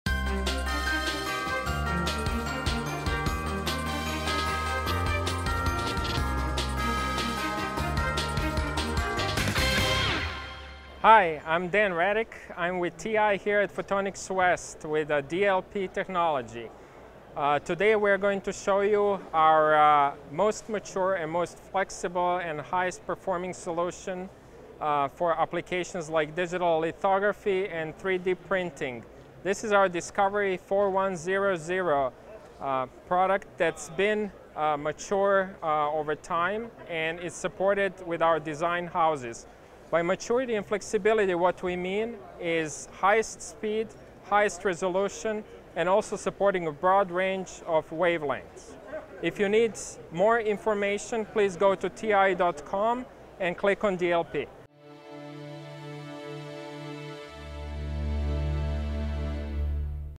DLP® 4100 high performance development platform
The DLP® Discovery™ 4100 is a high performance and highly flexible development kit for incorporating DLP technology into a wide variety of light steering applications. This high performance includes exceptionally fast pattern rates for light exposure and image capture that enable competitive cycle times in industrial markets.  At the heart of DLP technology is the Digital Micro-mirror Device (DMD). The Discovery 4100 is well suited for solutions requiring high resolution, optical throughput and brightness. Furthermore, the included DMD controller, DLPC410, offers designers maximum flexibility to format and sequence light patterns due its compatibility with various user-selected processors or FPGAs.  The DLP Discovery 4100 also offers designers more advanced control of the micro-mirrors through random row addressing in addition to full frame input. This added flexibility enables diverse architectures for industrial, medical, security, telecom, and instrumentation applications.